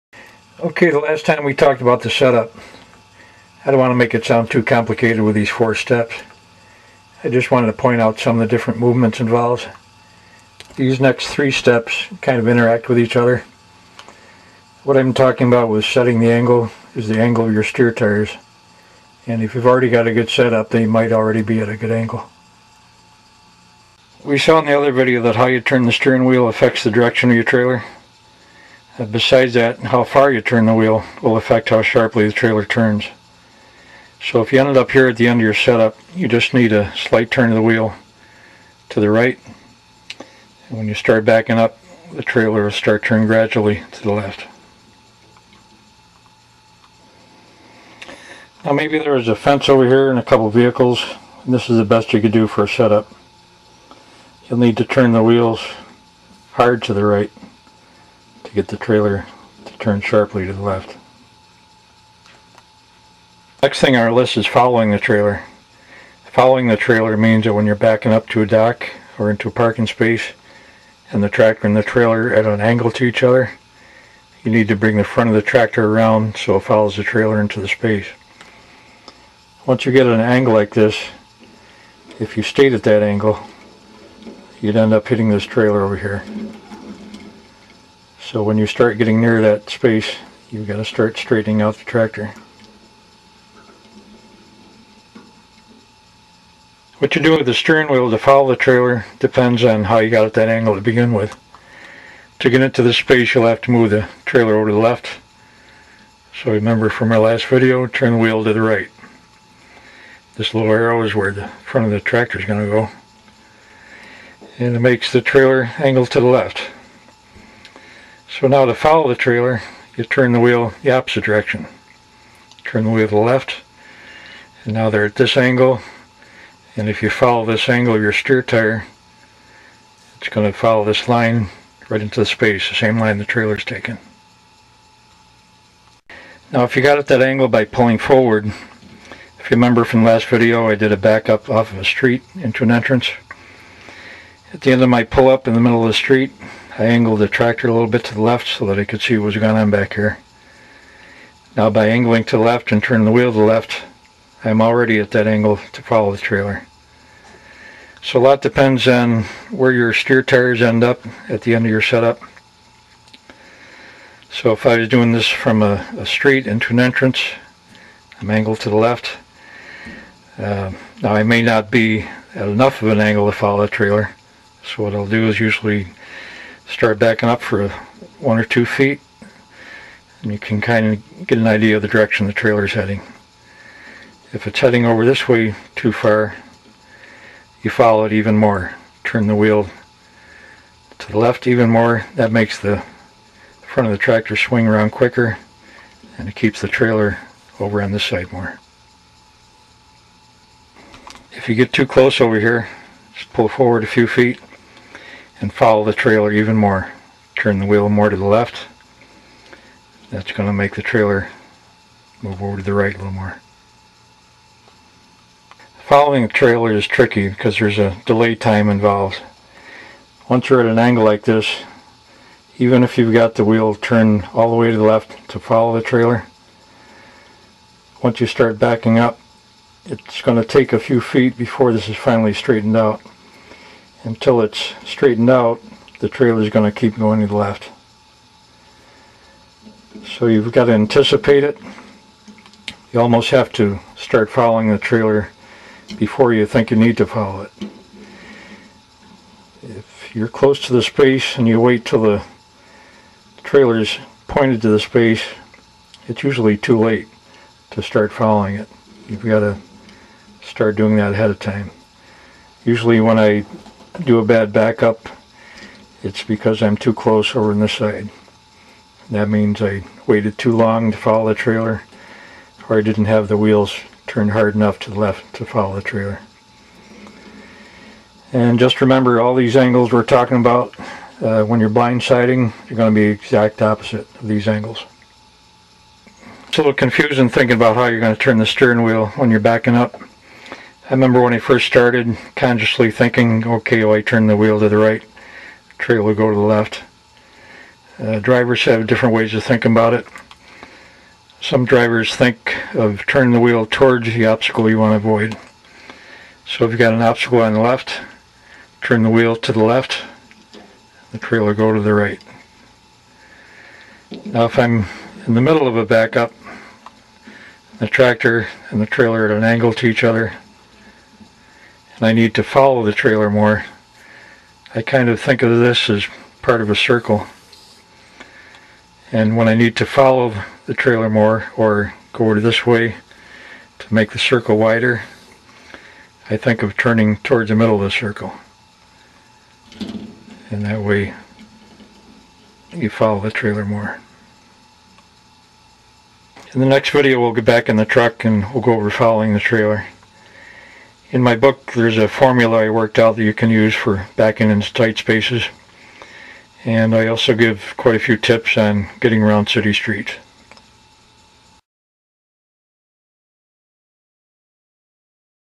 How to back up a tractor trailer pt. 3
This video covers angles and following the trailer